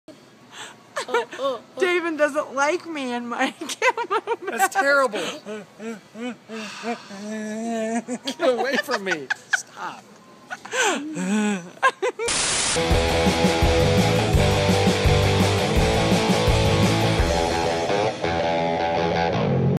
" The Camo Mask" (redneck Gimp mask)
Ana wears a terrifying camo mask and tries to seduce Davin. She looks more like "The gimp" from a Redneck version of "Pulp Fiction".